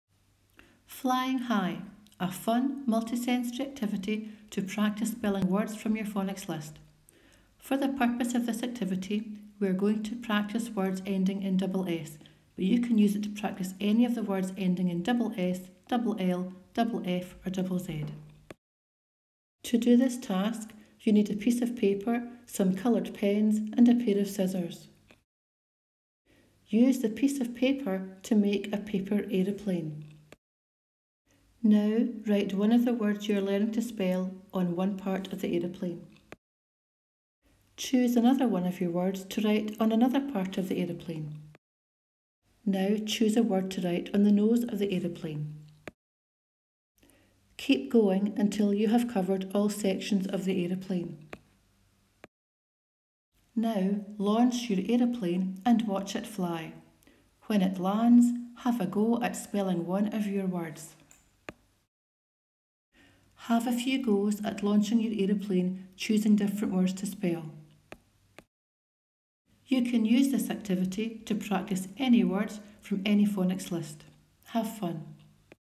Flying High
A multi sensory activity to practise spelling words ending in ss, ll, ff, zz.